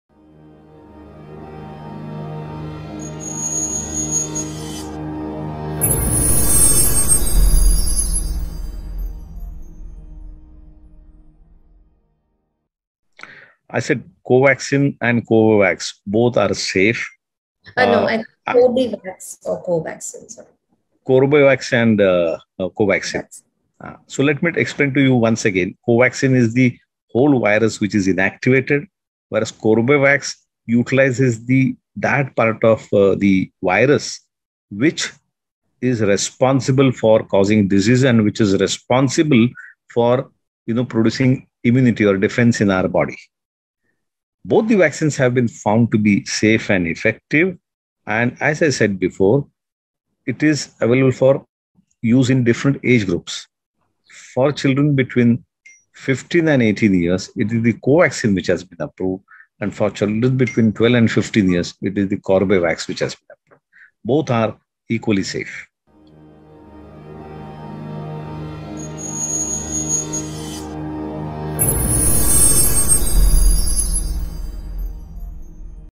Apollo Hospitals | Which vaccine is better: Corbevax or Covaxin? | Dr. Vijay Yewale
Watch the insights on "Which vaccine is better Corbevax or Covaxin?" by - Dr. Vijay Yewale, Consultant, Pediatrics, Apollo Hospitals.